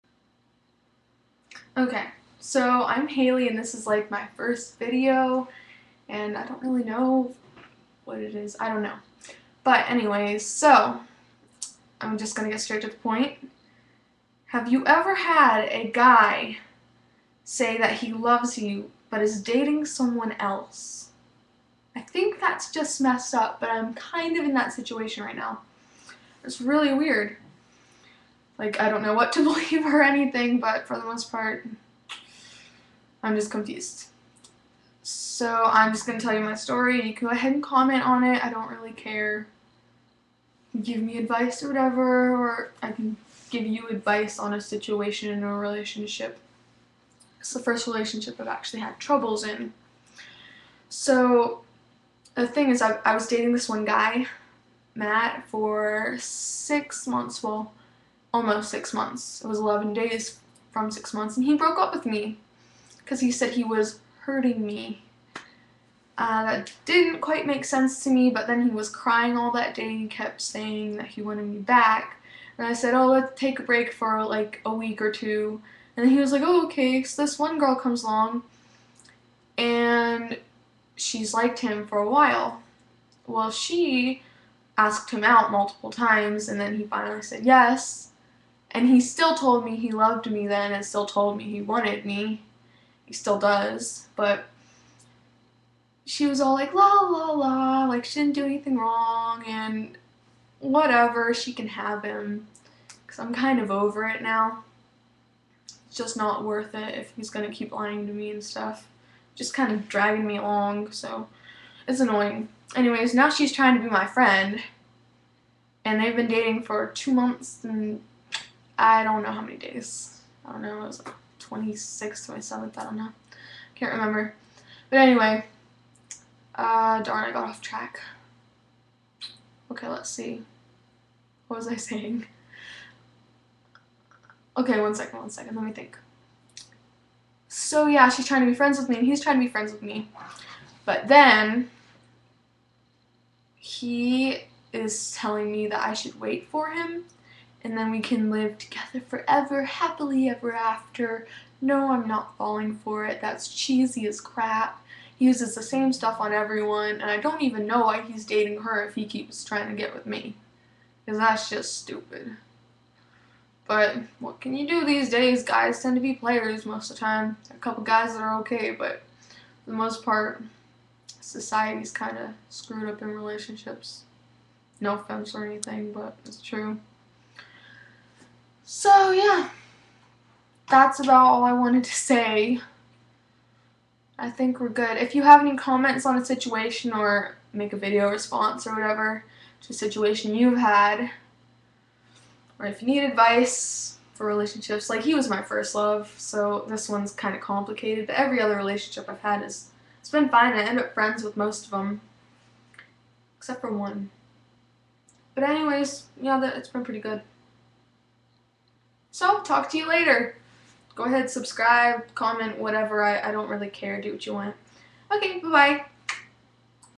Have You Ever Been in a Love Triangle?
Ok.. So Basically. I need you're opinion. Should I ignore this guy or should I listen to what he says & try to me friends.